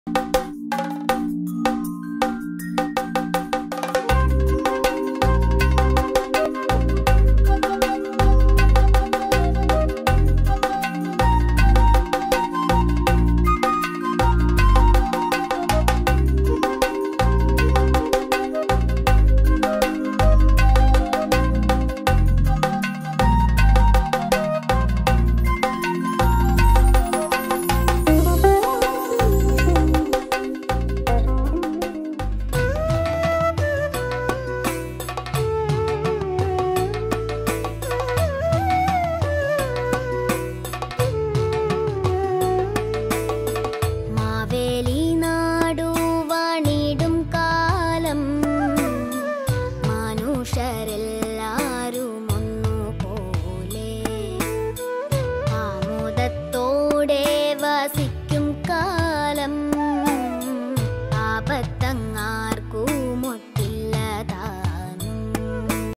Happy Onam 2023;SG The Reporter
Onam is the biggest festival in the Indian state of Kerala. Onam Festival falls during the Malayali month of Chingam (Aug - Sep) and marks the homecoming of legendary King Mahabali. Carnival of Onam lasts for ten days and brings out the best of Kerala culture and tradition. Intricately decorated Pookalam, ambrosial Onasadya, breathtaking Snake Boat Race and exotic Kaikottikali dance are some of the most remarkable features of Onam - the harvest festival in Kerala;SG The Reporter